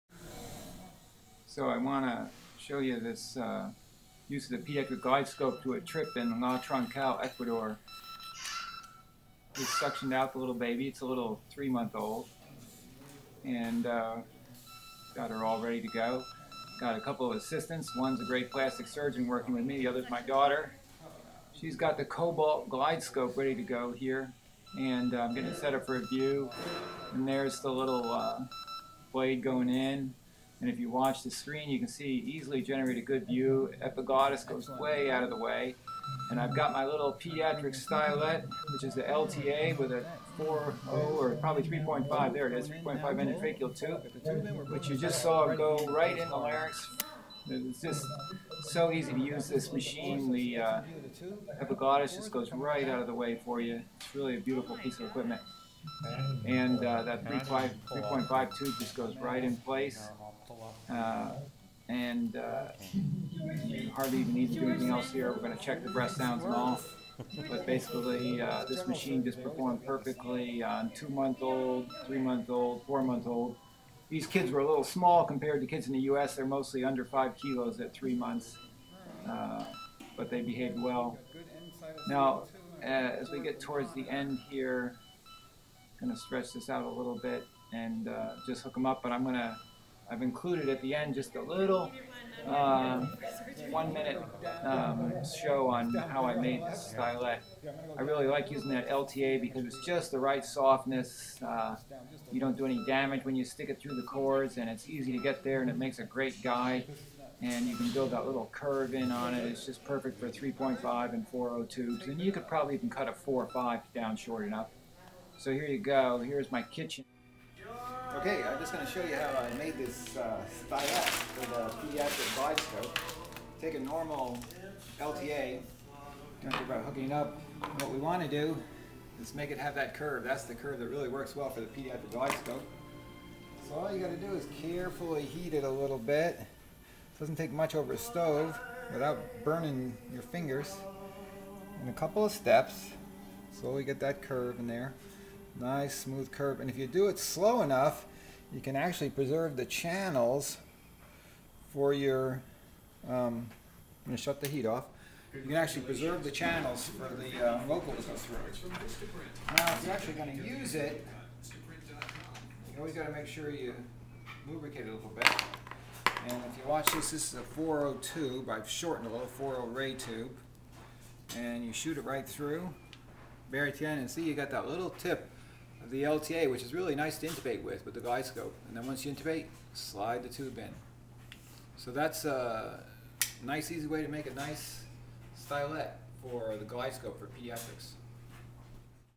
Pediatric Glidescope in Infants with LTA Stylet
Using the Cobalt Glidescope in a Pediatric patient durting a trip to La Troncal, Ecuador March 2011. Filmed by Susan Doolittle, assistant Katie Doolittle, Gasser Gary Doolittle. Many Thanks to the Verathon Company and Brian Roberts for loaning me the Glidescope Cobalt for this mission trip to Ecuador. Don't leave home without it!